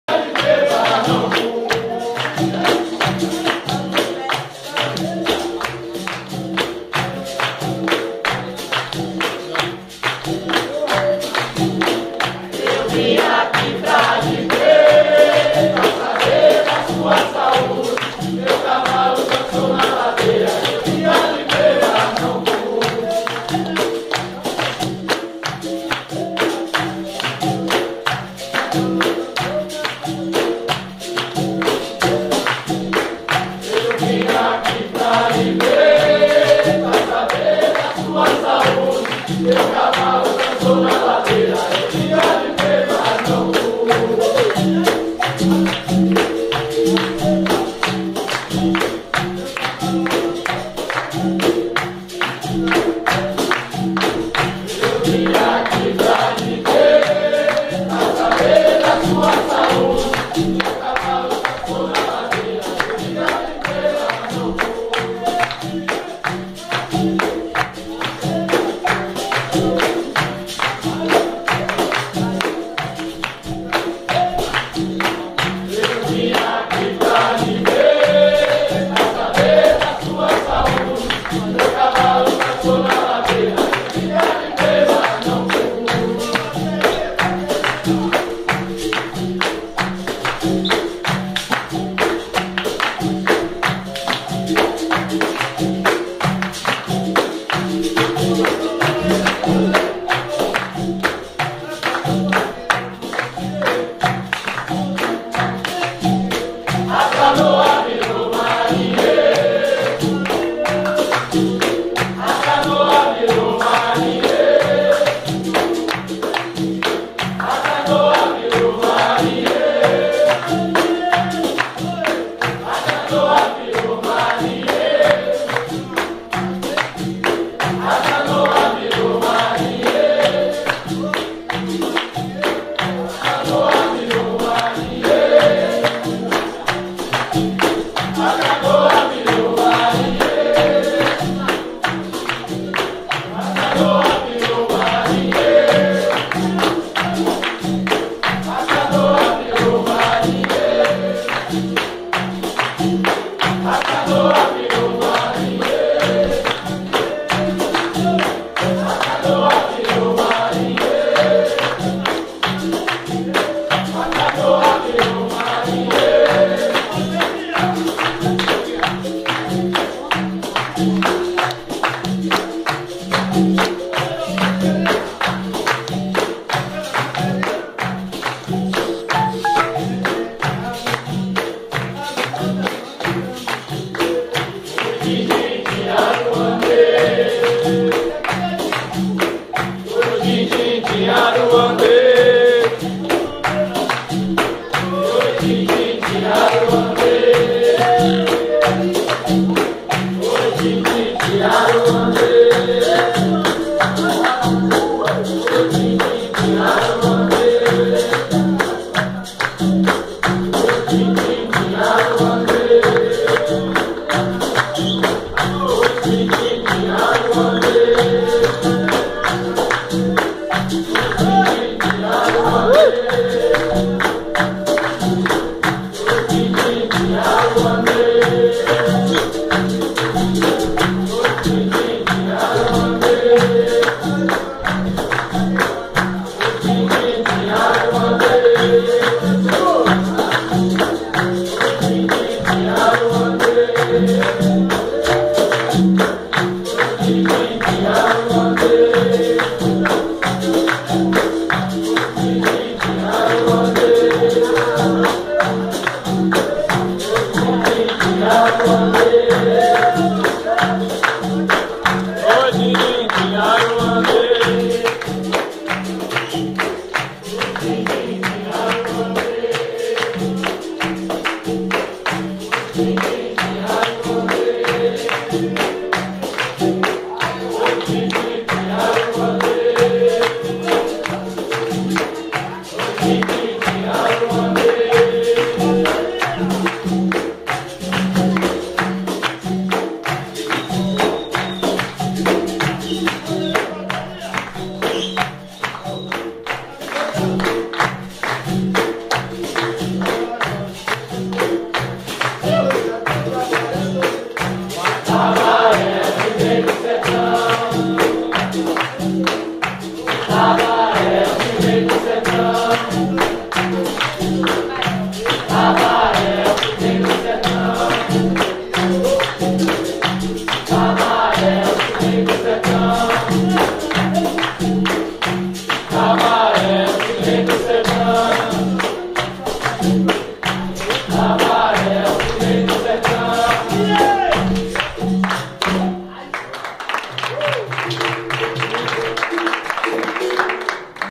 Prof. Tiziu e C. M Gugu
Da série - Jogando Capoeira

É mandinga!

E mais uma vez com meu mano.
@Guguquilombola, gratidão pelo belo jogo🙏🏾🙏🏾🙏🏾.

Jogando ao som do grande mestre Ananias.

Deixem seu comentário no vídeo

Gratidão 🙏🏾.